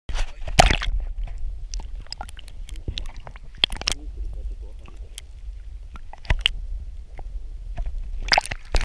カヤックフィッシングで釣れた 鯛の水中映像 pesca o peixe vermelho
カヤックフィッシングで釣れた鯛
ストリンガーにて水中映像